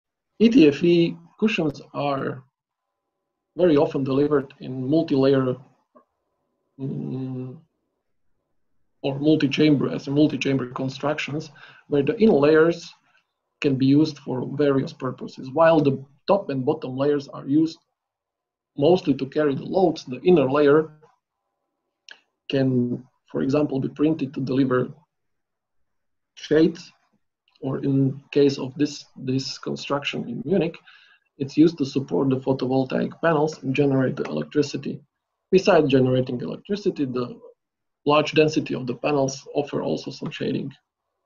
Three layered ETFE film cushions with photovoltaic cells (AWM Carport Munich, Germany)
Dr Peter Novysedlák from Taiyo Europe GmbH (Taiyo Kogyo) talks to us about the use of three layered ETFE film cushions with photovoltaic cells in the roof of the AWM Carport Munich (Munch, Germany).